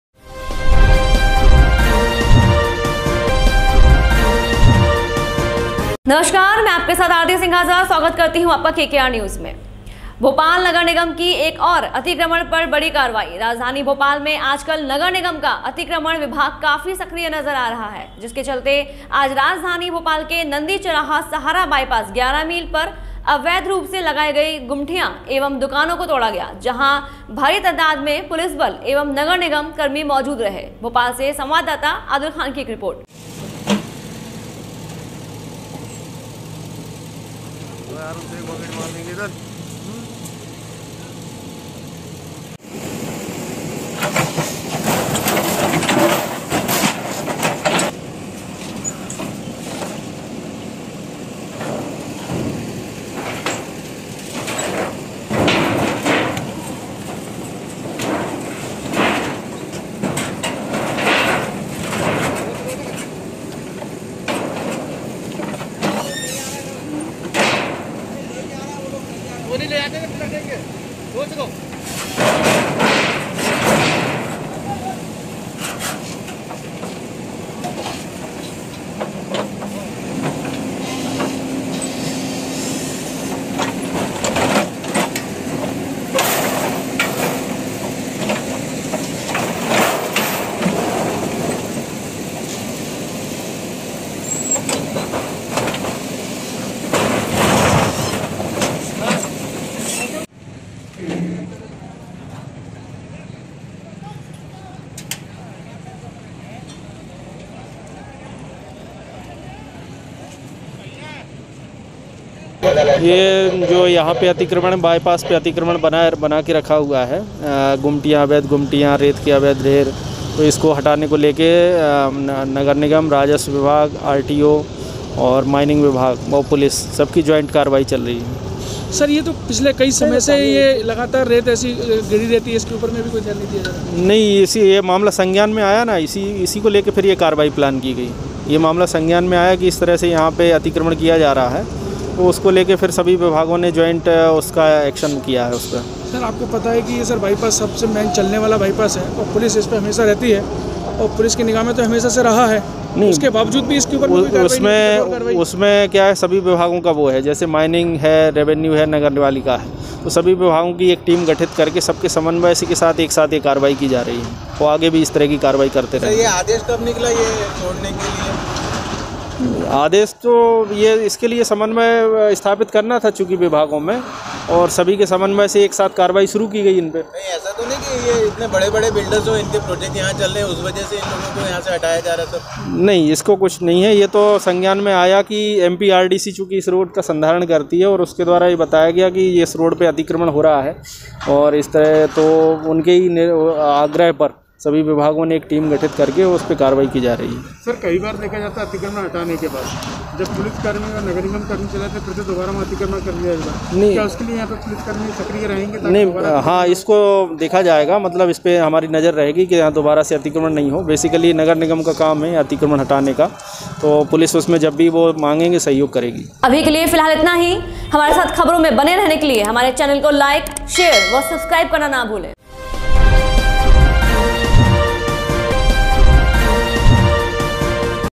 भोपाल नगर निगम की एक और अतिक्रमण पर बड़ी कार्रवाई।
राजधानी भोपाल में आजकल नगर निगम का अतिक्रमण विभाग काफी सक्रिय नजर आ रहा है जिसके चलते आज राजधानी भोपाल के नंदी चौराहा सहारा बाईपास 11 मील पर अवैध रूप से लगाई गई गुमठियां एवं दुकानों को तोड़ा गया जहां भारी तादाद में पुलिस बल एवं नगर निगम कर्मी मौजूद रहे

 भोपाल से संवाददाता आदिल खाँन की एक रिपोर्ट।